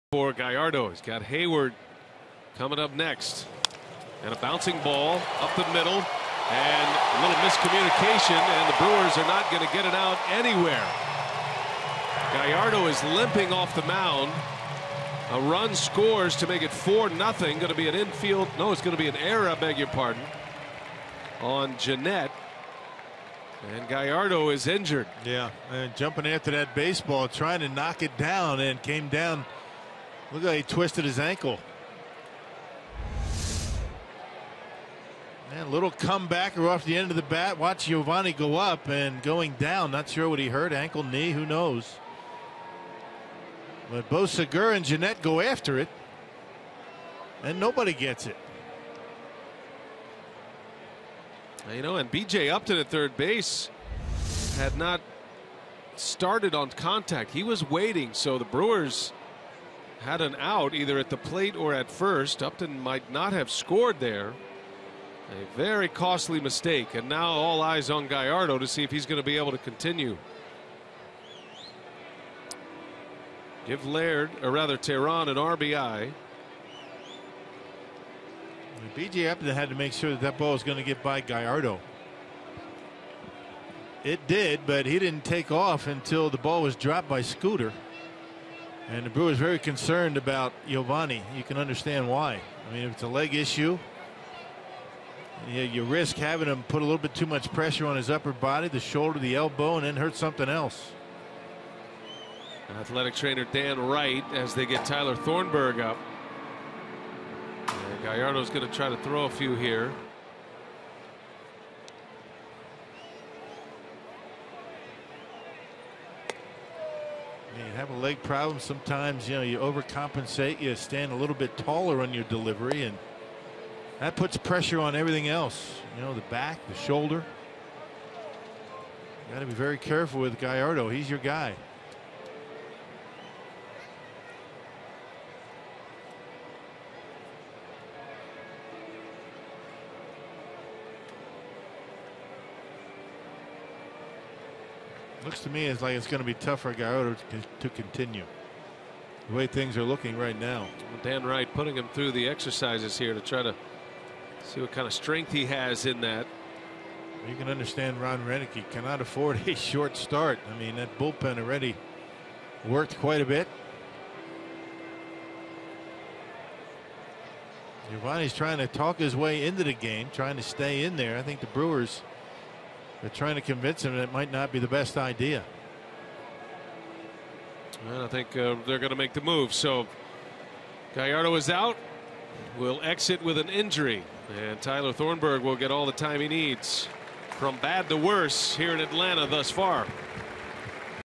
MIL@ATL: Gallardo hurt on grounder, leaves game
5/20/14: Yovani Gallardo is injured trying to field a grounder, leaving the game after being checked by the trainers